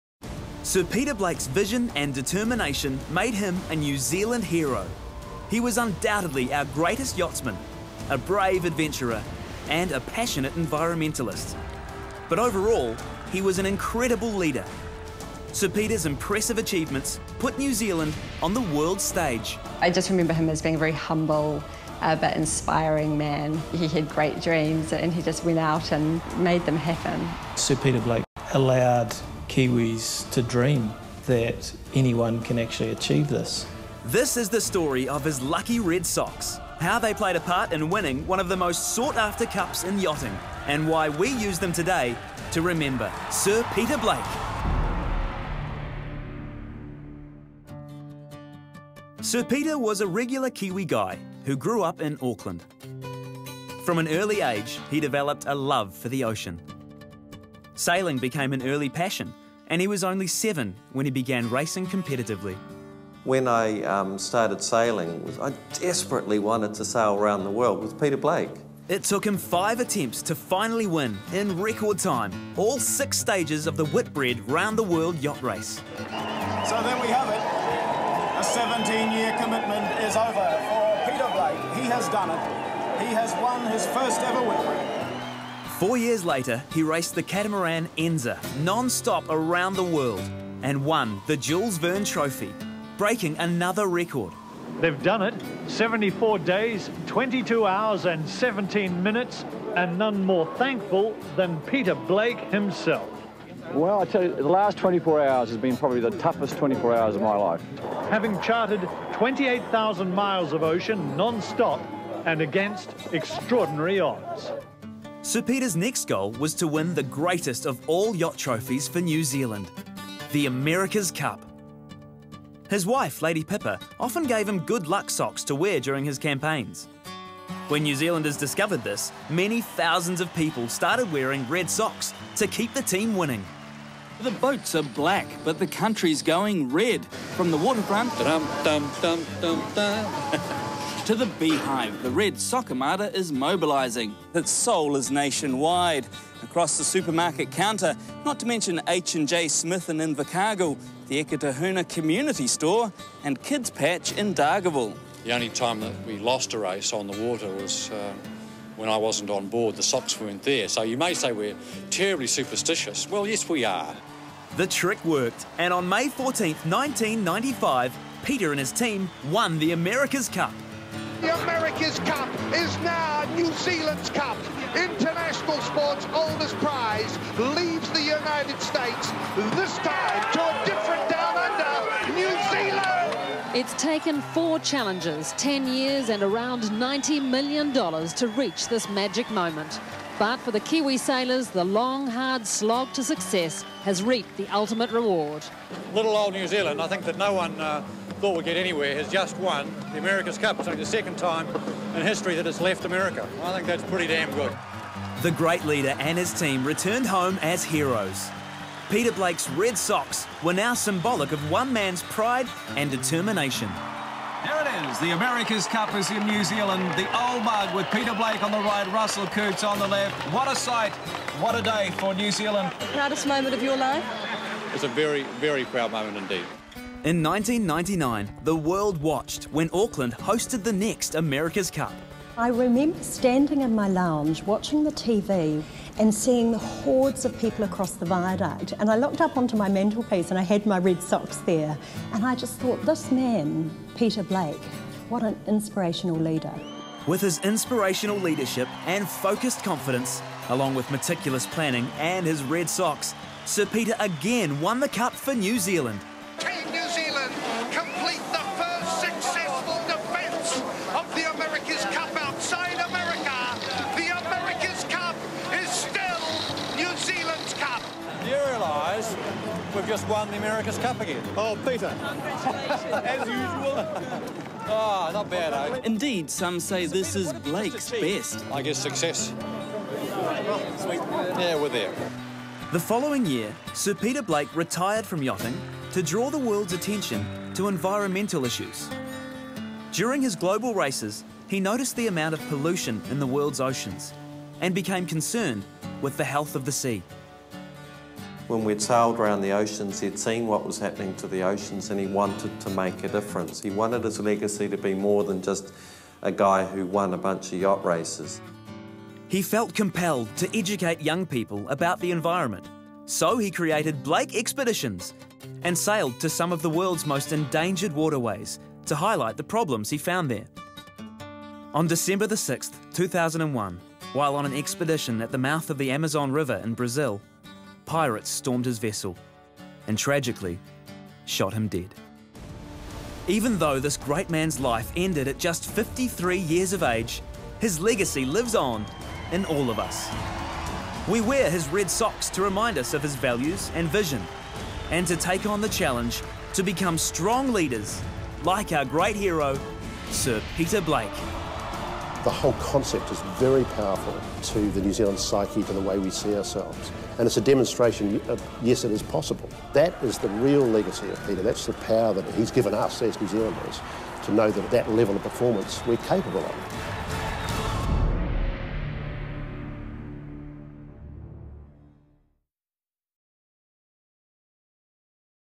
Famous Red Socks
The story of Sir Peter Blake's famous Red Socks!

This film is part of the Sir Peter Blake Trust's Lessons in Leadership series of DVDs. For more information on the Trust and Leadership Awards: